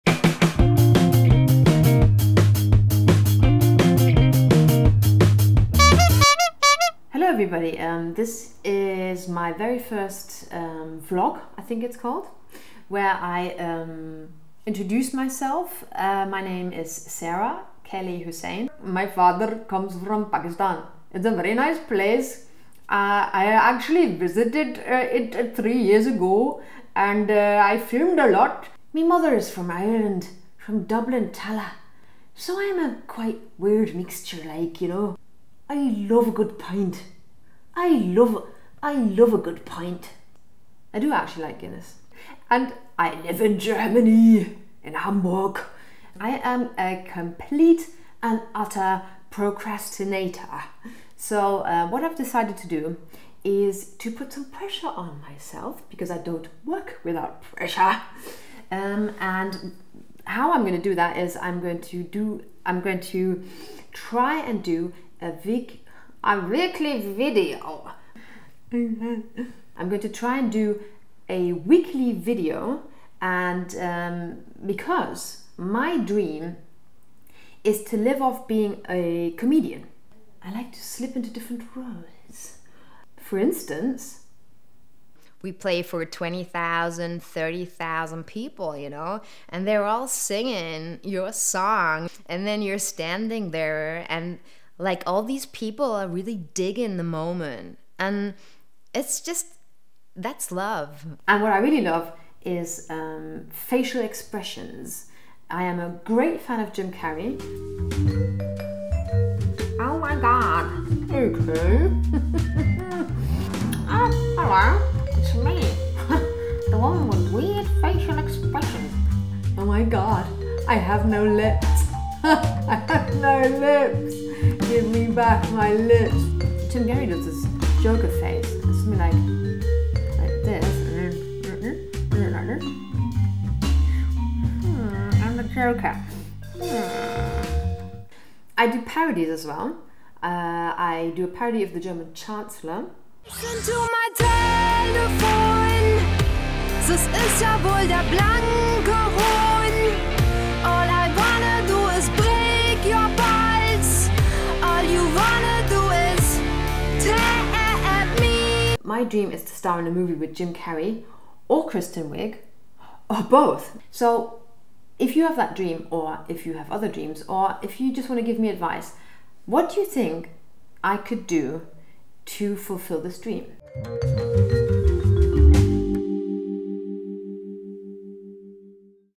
Kellyvision - I have a dream
This is my very first vlog entry. My dream is to become a full-time comedian actress. What are your dreams? How do you pursue them?